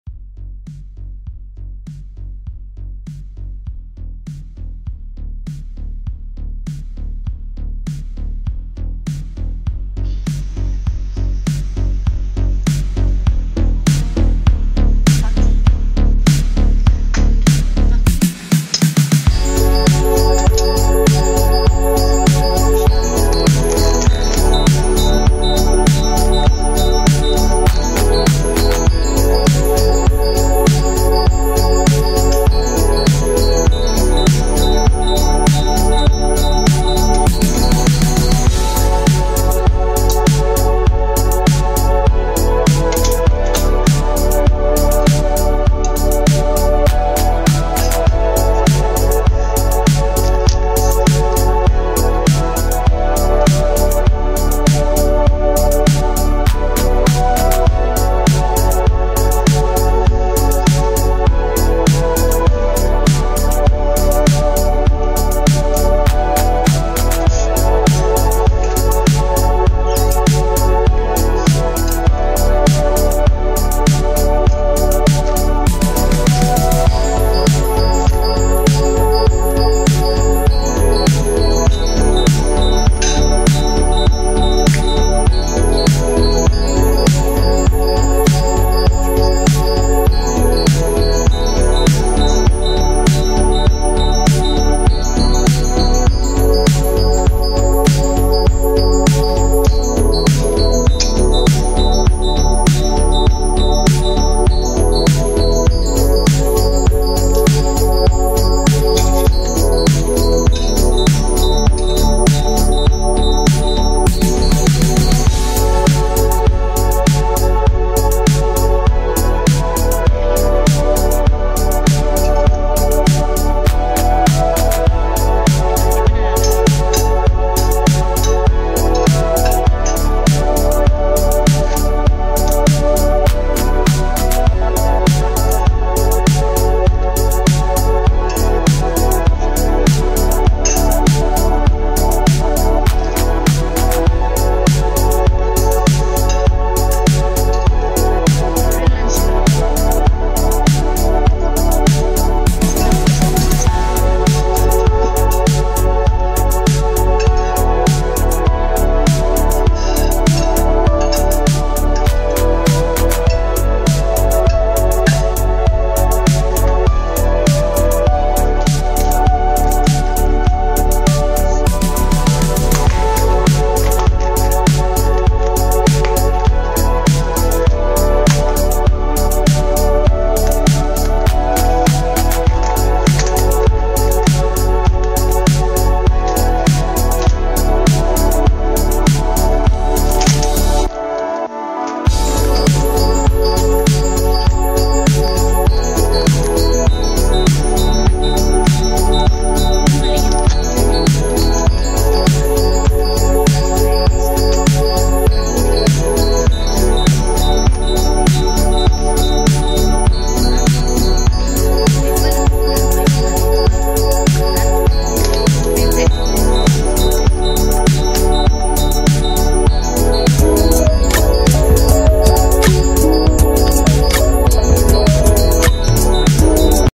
Weekly Baon ni Mae# Healthy Living
always try our healthy food for lunch box of our children economical and easy to cook. Try it 🍅🍒